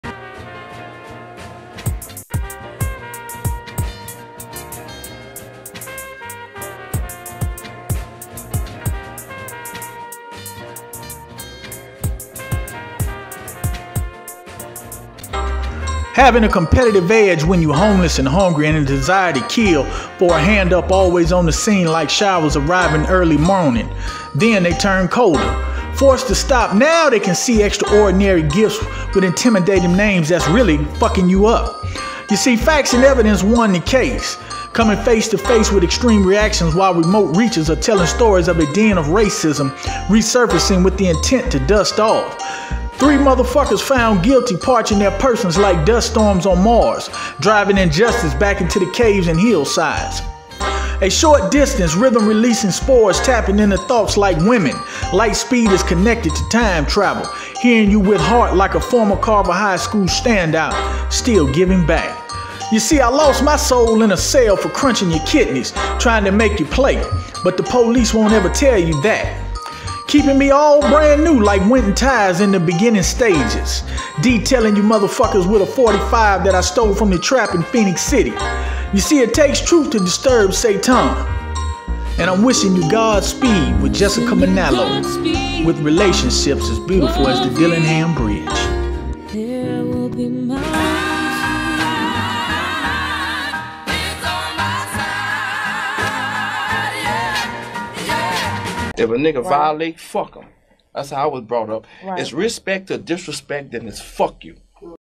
Truth Awakens Satan!
Having a competitive edge when you homeless and hungry and a desire to kill for a hand-up always on the scene like showers arriving early morning then turning colder

Forced to stopped now they can see extraordinary gifts with intimidating names that fucking you up!

Facts and evidence won the case Coming face to face with extreme reactions while remote reaches are telling stories of a den of racism resurfacing with the intent to dust-off; found Guilty

Parching their person like dust storms on Mars driving injustice back into the caves and hillsides; A short-distance

rhythm releasing spores tapping into thoughts like women, light-speed is connected to time-travel hearing with your heart like a former Carver High school standout still giving back;

You see, I lost my soul in a cell for Crunching your kidneys trying to make you play, but the police won't ever tell you that keeping me all brand new like Wynnton tires in the beginning stages

Detailing them with a .45 that I stole from the trap in Phenix City, It takes truth to disturb Satan

Wishing you Godspeed with @jessicamanalo with relationships as beautiful as the Dillingham bridge.